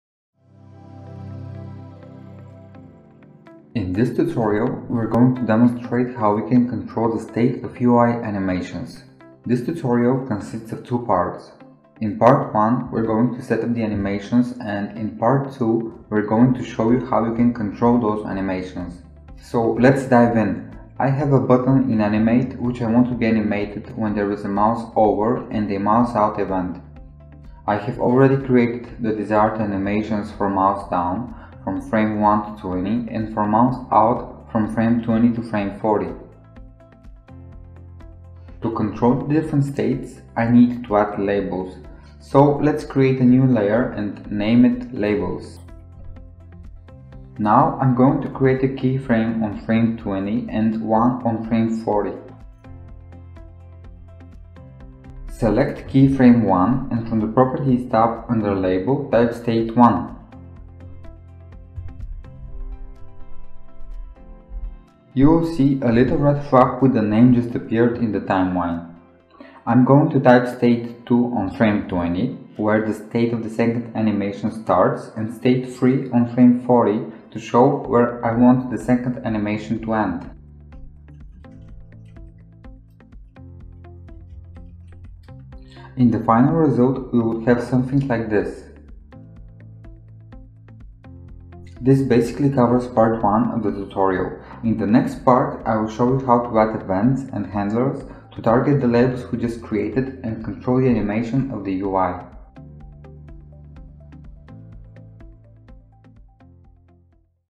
How to control the state of UI animations (Part 1)
How you can control different states of your UI animations?

This 2-part tutorial demonstrates how to set up and control animations in Adobe Animate.